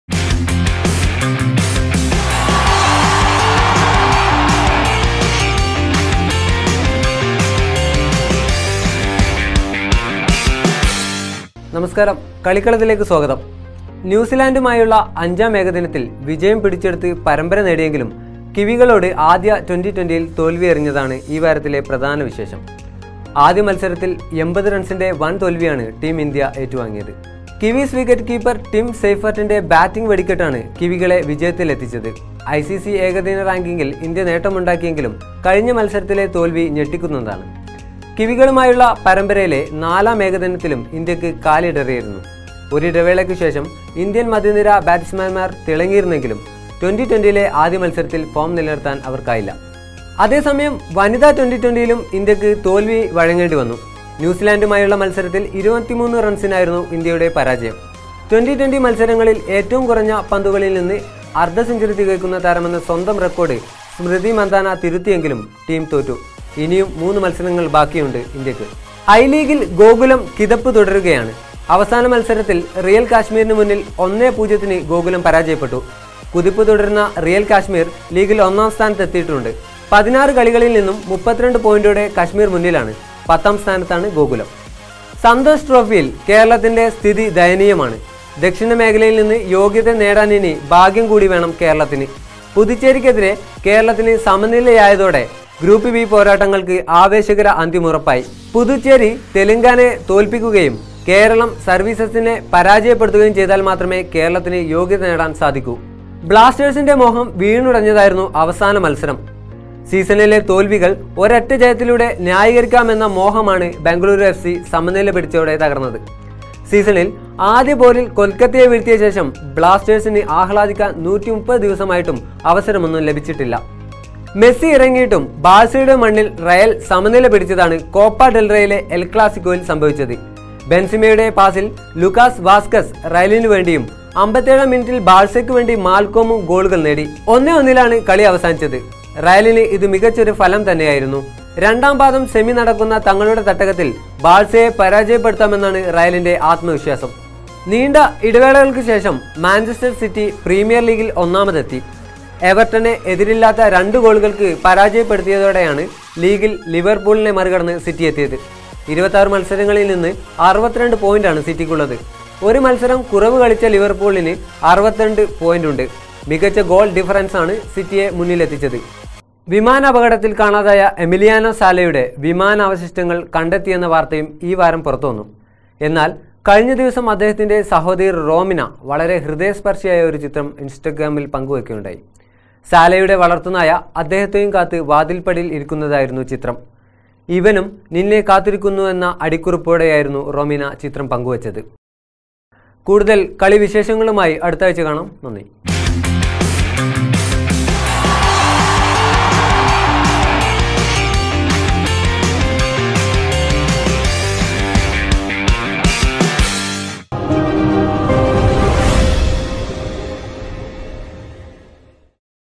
ബാഴ്‌സയുടെ മണ്ണിൽ റയലിന് നേട്ടം |THEJAS NEWS | KALIKKALAM Episode 5
* ഇടവേളകള്‍ക്കു ശേഷം മാഞ്ചസ്റ്റര്‍ സിറ്റി പ്രീമിയര്‍ ലീഗില്‍ ഒന്നാമതെത്തി.
* എമിലിയാനോ സാലയുടെ വിമാനാവശിഷ്ടങ്ങള്‍ കണ്ടെത്തി
* സന്തോഷ് ട്രോഫിയില്‍ യോഗ്യത നേടാന്‍ കേരളത്തിന് ഭാഗ്യംകൂടി വേണം